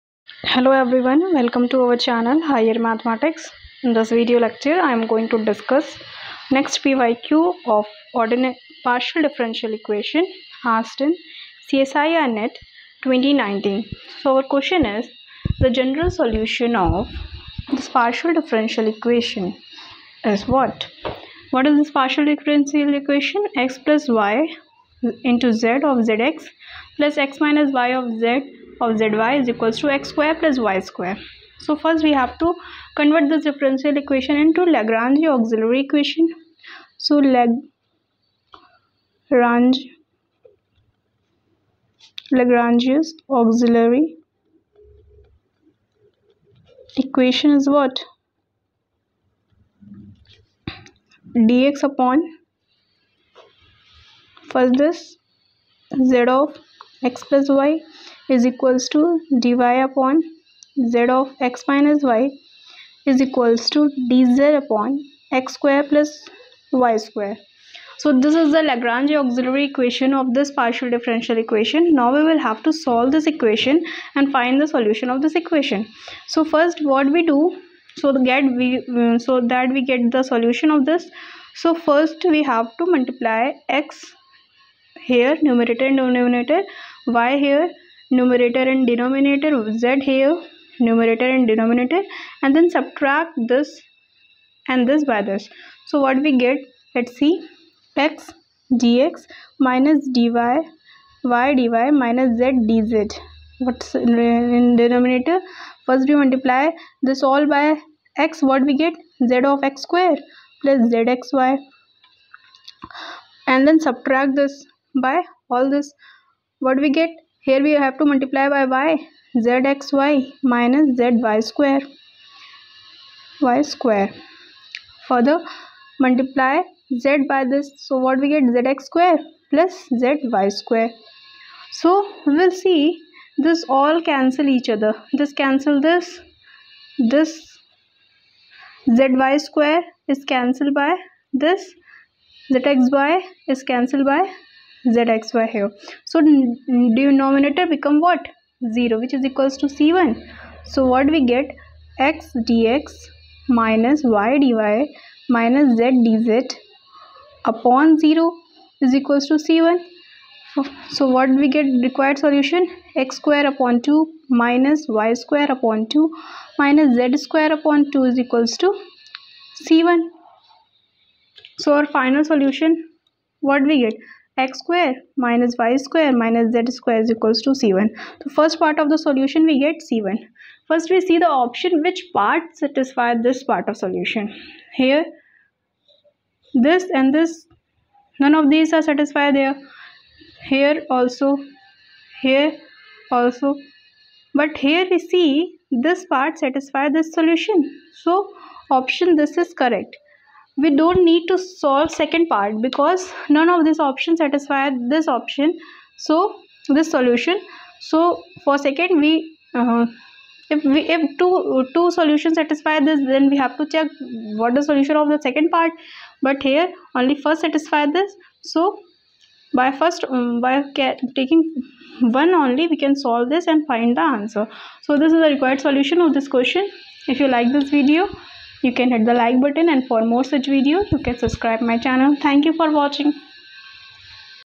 Partial differential equations|| csirnet 2019.
This lecture explains you the PYQs on Partial differential equations and helps you in clearing your concepts.@Mathematics_Class4